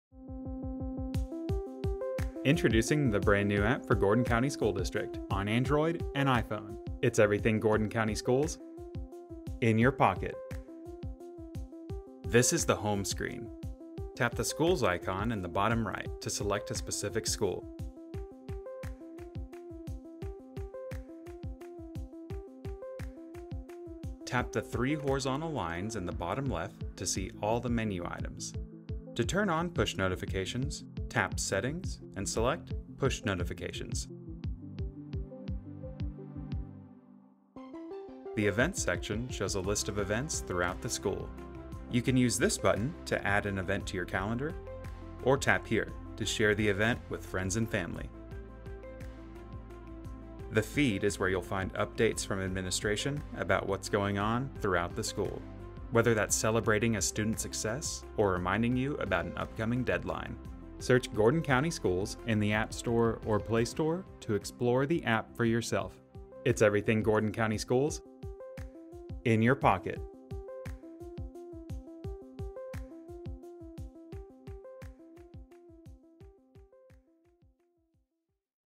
Gordon County Schools New Mobile App
Gordon County Schools is thrilled to announce our new app
for mobile phones! Stay up-to-date with access to events,
documents, news from administrators, student stories, and
even emergency notifications.